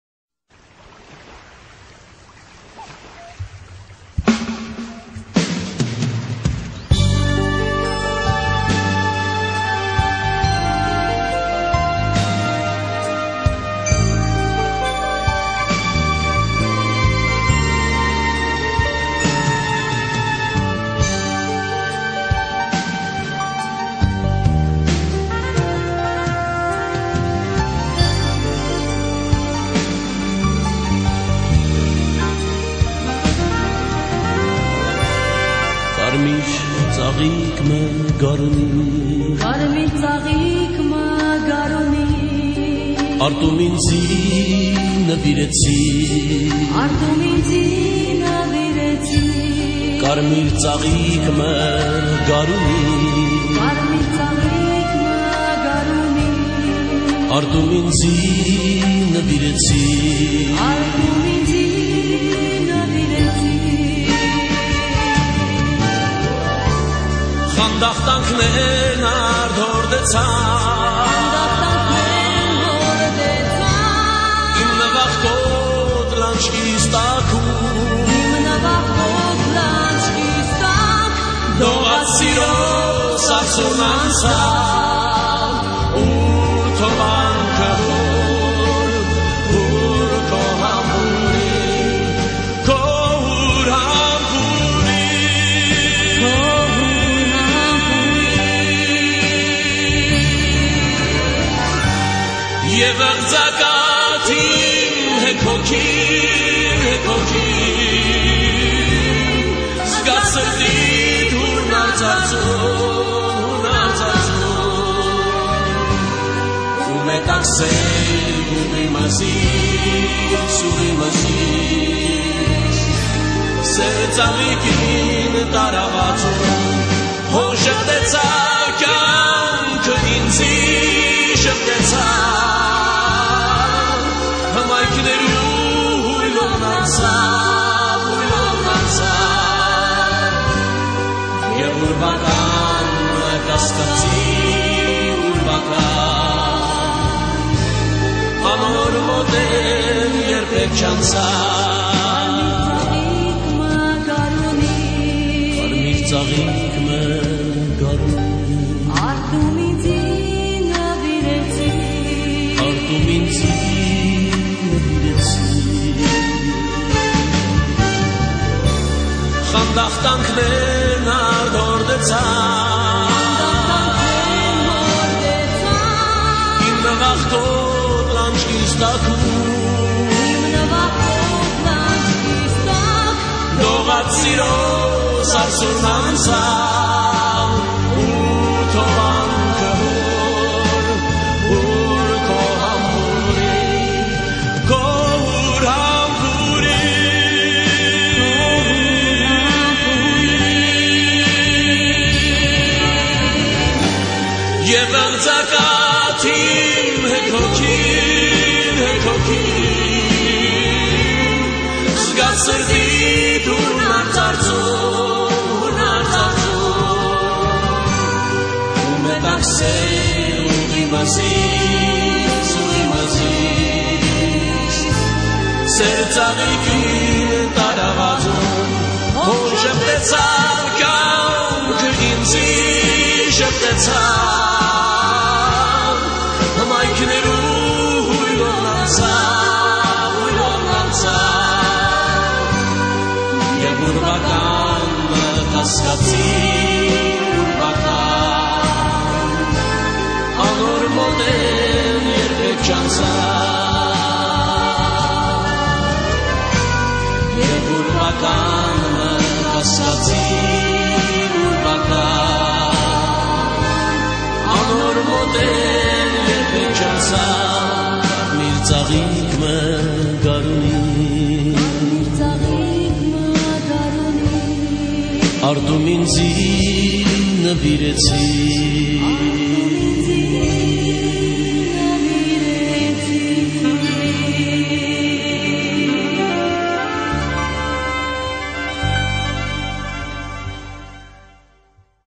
Nairian Girls/Armenian Girls/Армяночки
These are ordinary Armenian girls who got their photos taken by photographers, mamy of who are beginners in photography ;-)